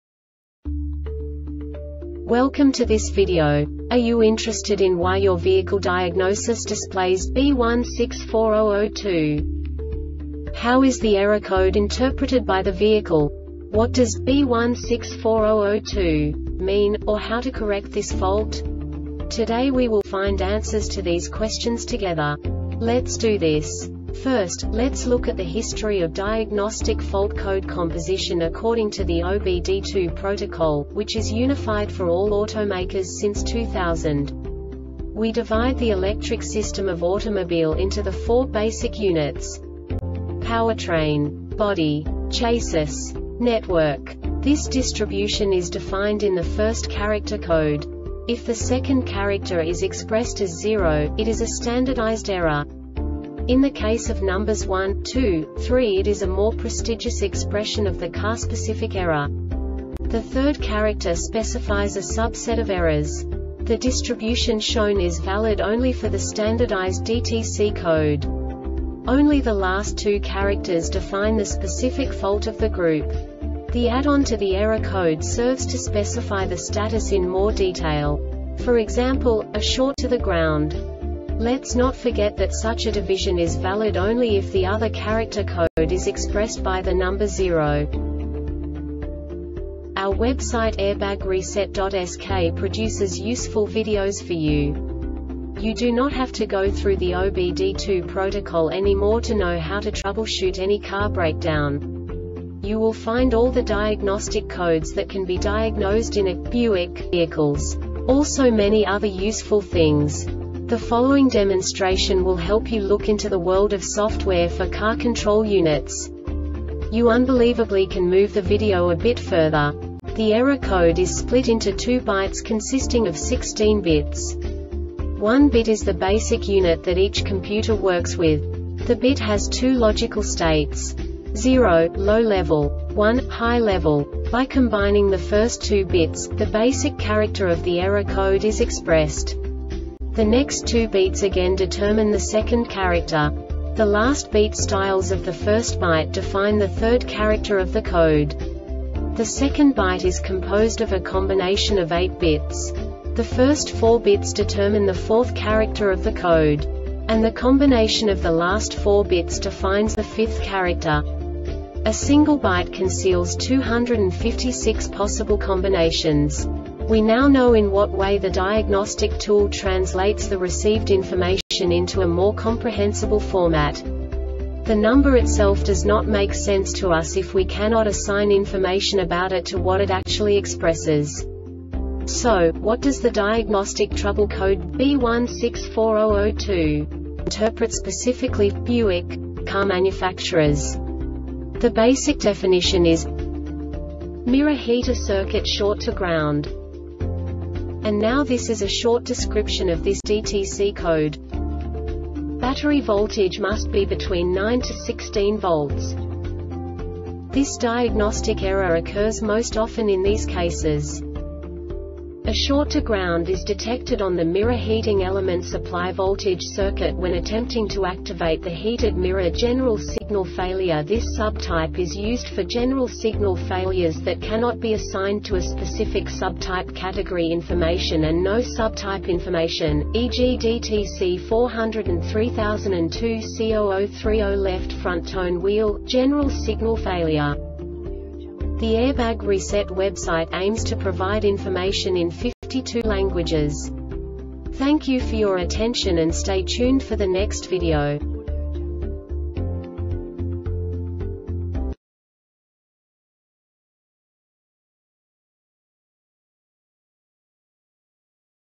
DTC Buick B1640-02 Short Explanation
Contents:
0:21 Basic DTC analysis according to OBD2 protocol standard.
1:48 Insight into programming
2:58 A diagnostically understandable description of the Diagnostic Trouble Code
3:05 Basic error definition

Make
Buick
Code
B1640 02
Definition
Mirror Heater Circuit Short to Ground
Description
Battery voltage must be between 9-16 volts
Cause
A short to ground is detected on the mirror heating element supply voltage circuit when attempting to activate the heated mirror
Failure Type
This sub type is used for General Signal Failures that cannot be assigned to a specific sub type Category information and no Sub Type information, eg DTC 403002 C0030 Left Front Tone Wheel - General Signal Failure